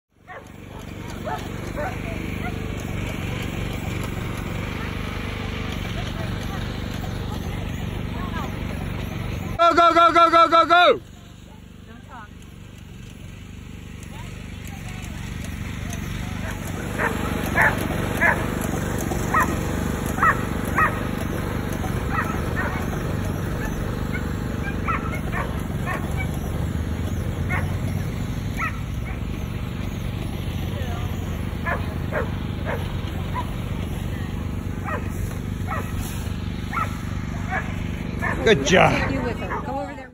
Catskill Fani Lure Coursing
Pumi Lure Coursing at AKC CAT event in Freehold, NJ